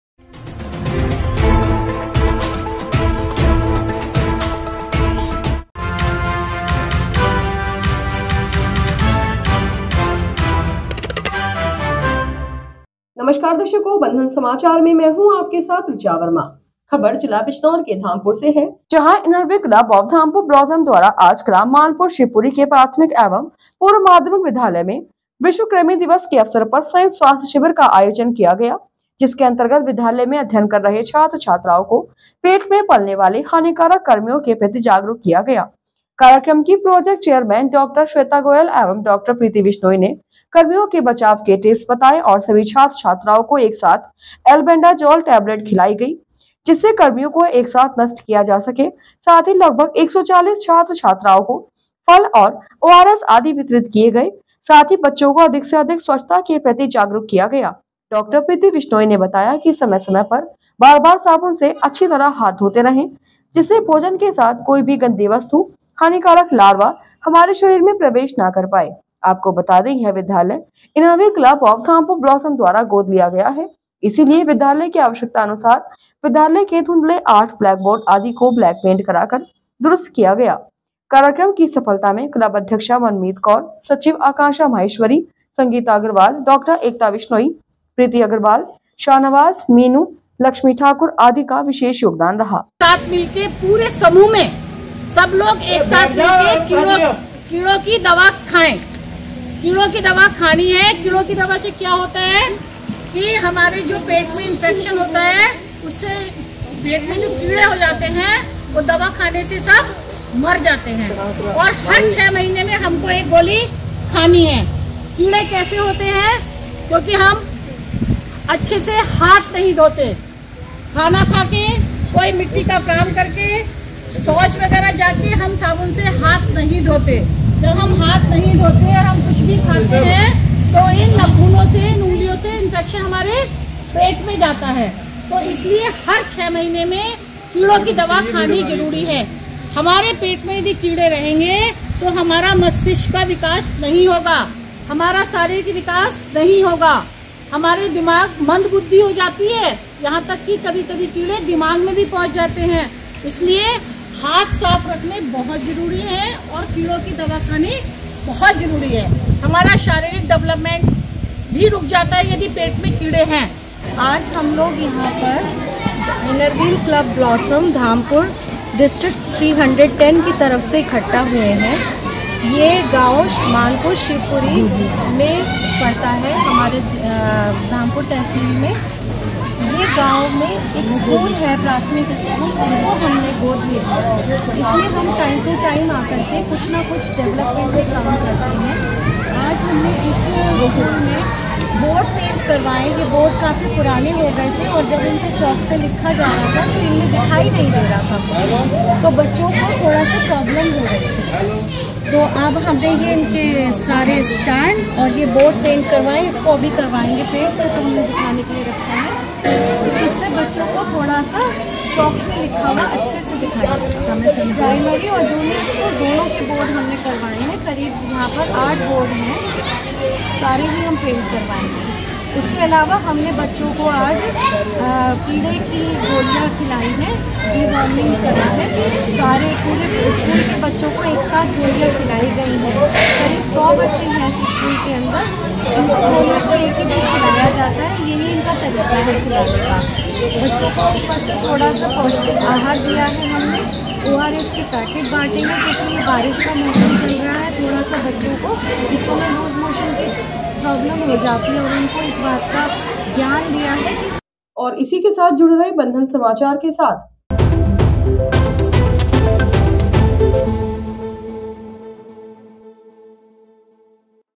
इनरव्हील क्लब ऑफ धामपुर ब्लॉसम द्वारा राष्ट्रीय कृमि दिवस पर लगाया गया संयुक्त स्वास्थ्य शिविर
इनरव्हील क्लब ऑफ धामपुर ब्लॉसम द्वारा आज ग्राम मानपुर शिवपुरी के प्राथमिक एवं पूर्व माध्यमिक विद्यालय में विश्व कृमि दिवस के अवसर पर एक संयुक्त स्वास्थ्य शिविर लगाया गया